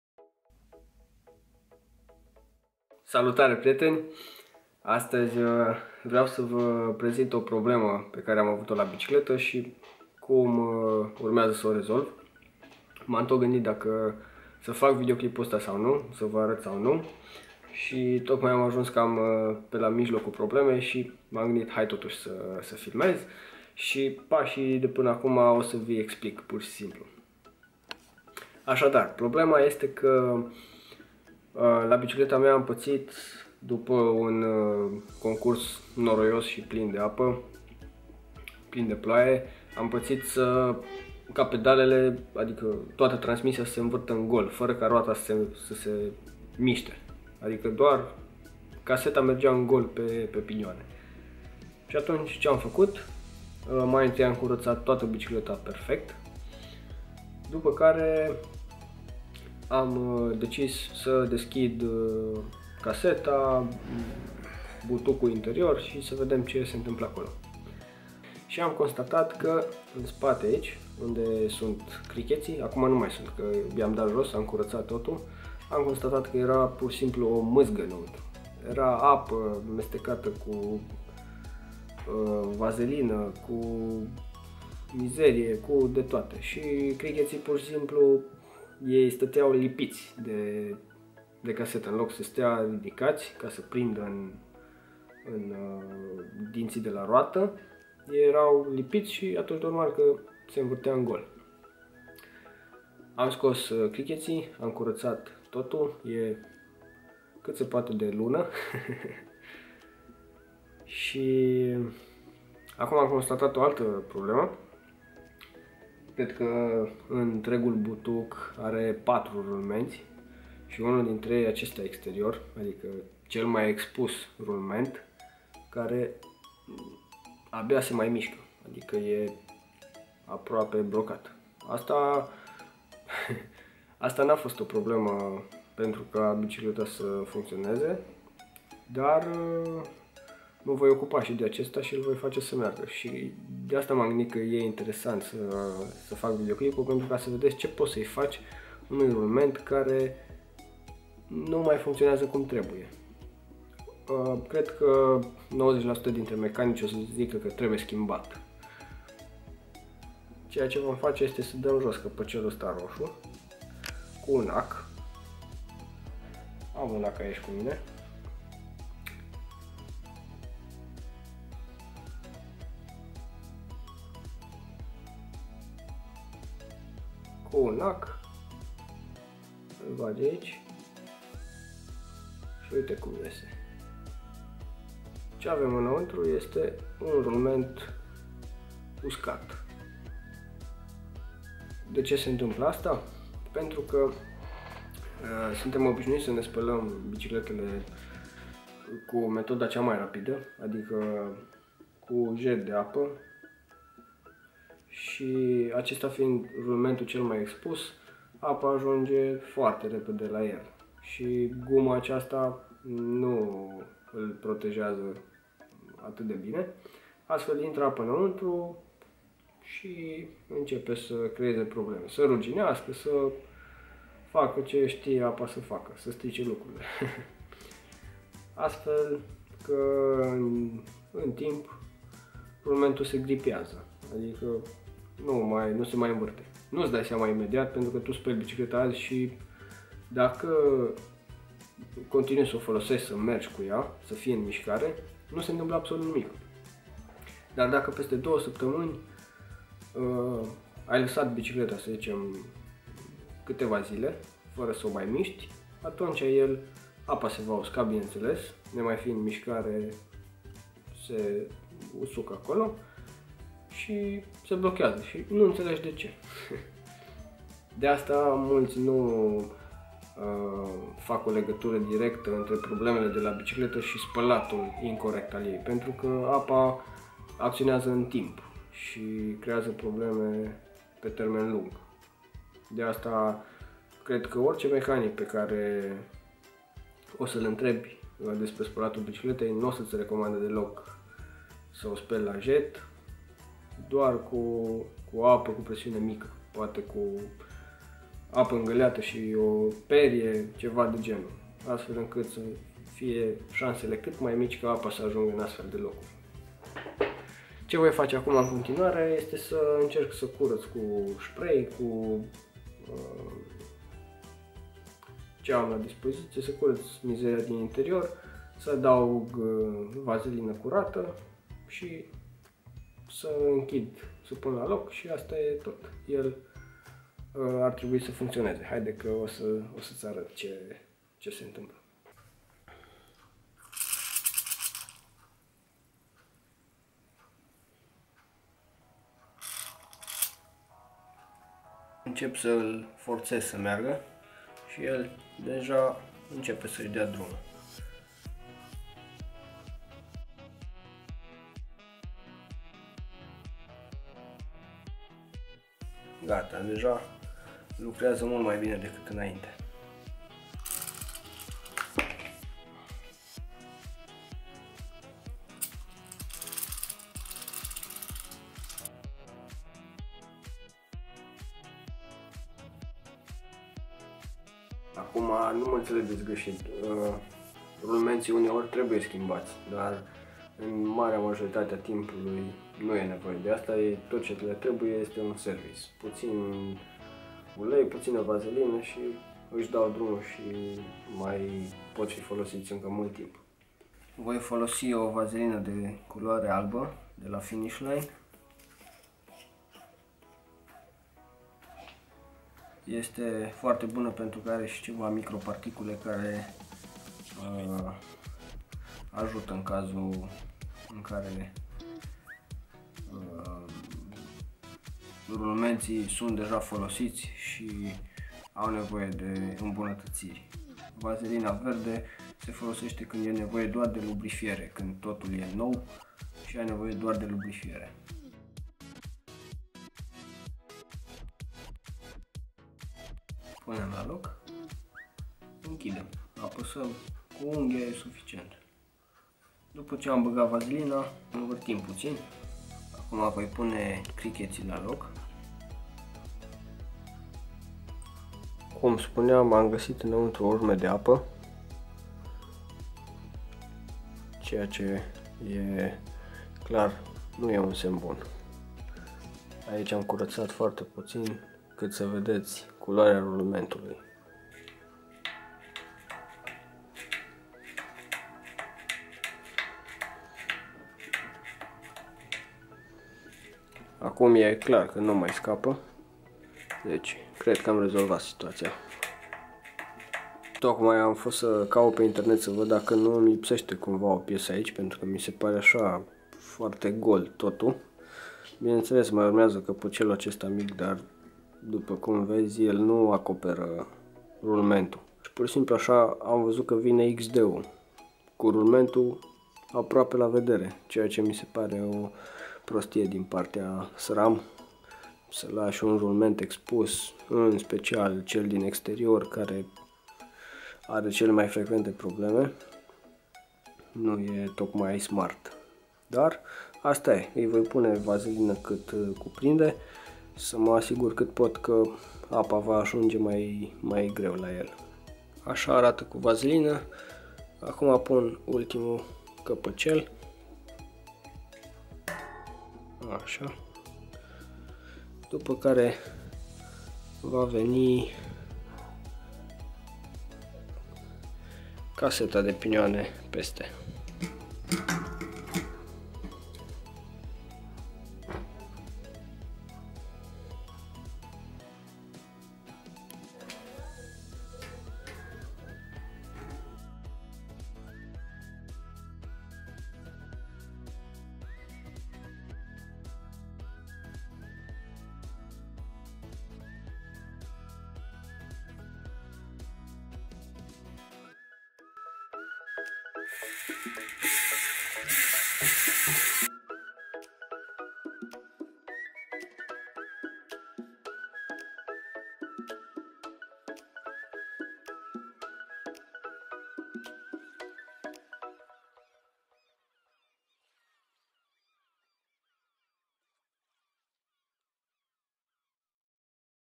Service Butuc la Bicicleta - Hub service
In acest video vei gasi raspunsul la mai multe intrebari:
1. Cum facService Butuc la Bicicleta - Hub service ?
2. Ce fac daca am un rulment blocat ?
3. Ce fac daca se invart pedalele in gol ?
4. Cum dau jos caseta de pinioane ?
5. Care este modul gresit de a spala bicicleta ?

Iti multumesc ca esti prezent si ca faci parte din povestea Giolive !
- Sergiu